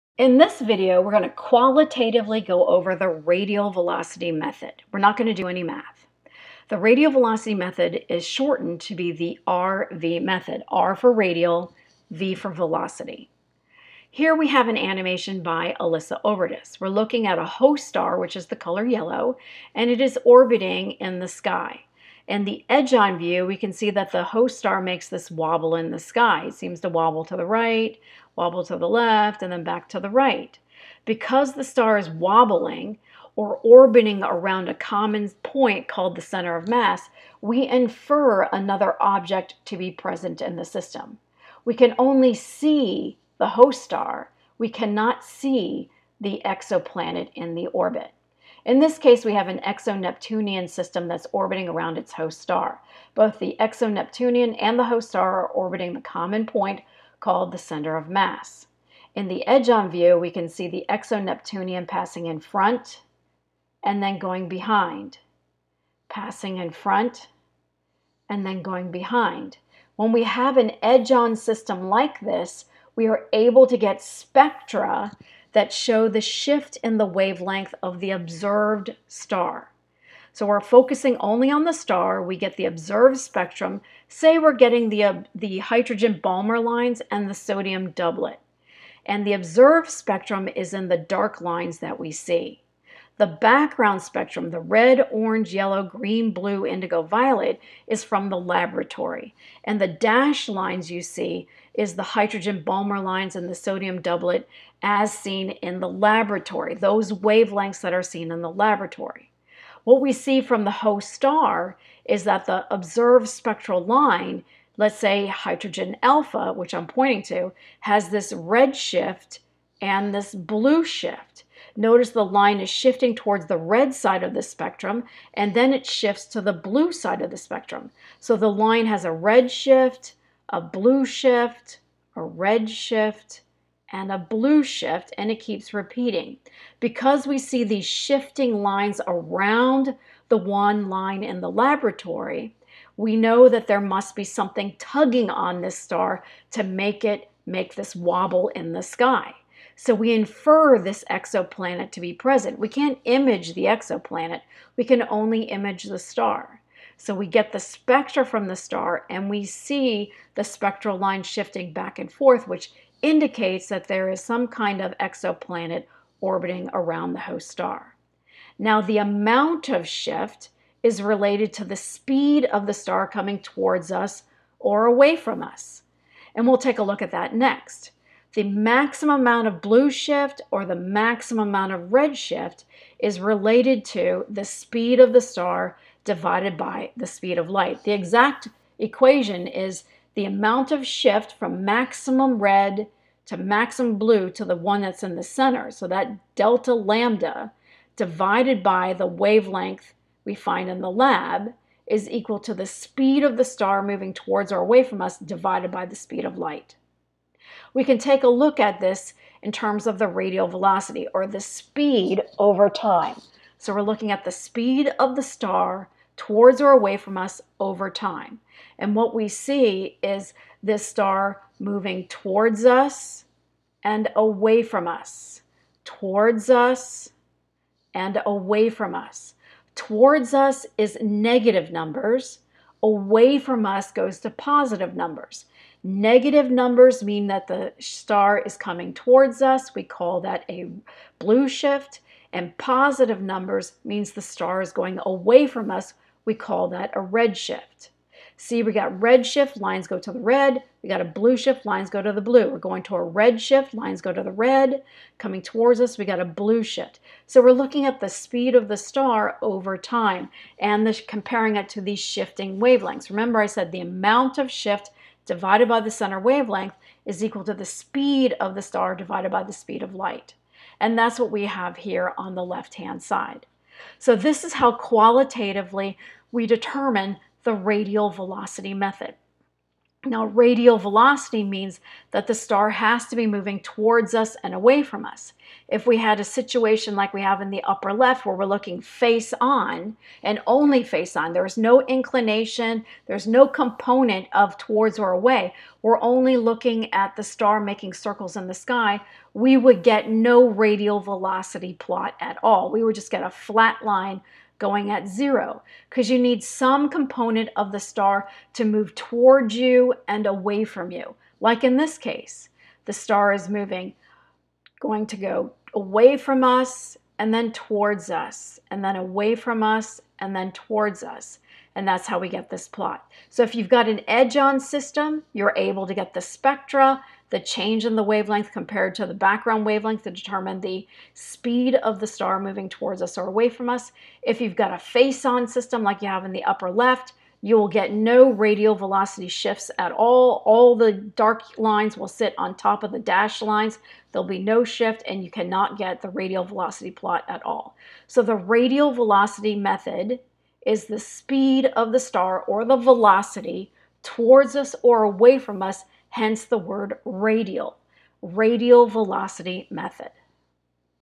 A Qualitative Explanation and Animation of the Radial Velocity Method
AstroPictionary Concepts
Astronomy Concepts